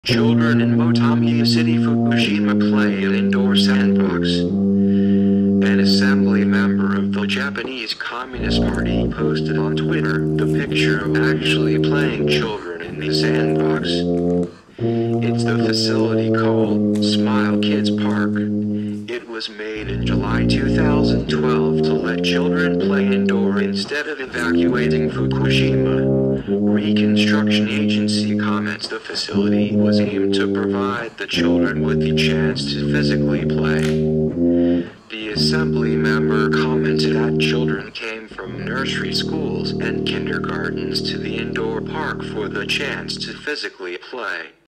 FUKUSHIMA sandbox
children forced to play indoors because of radioactive contamination in their neighbourhood